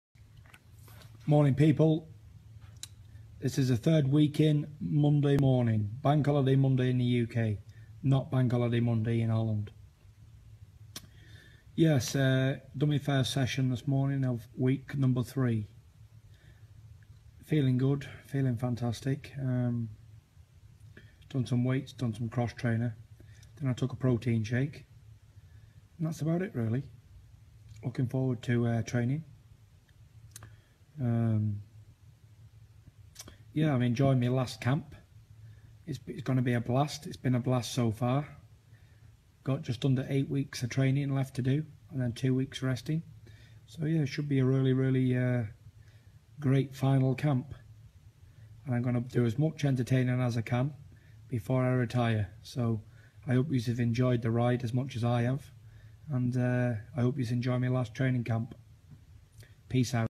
Tyson Fury's training camp diary - Week 3, Day 1 TRAINING CAMP 2016
"I'm going to do as much entertaining as I can before I retire, enjoy my last training camp!"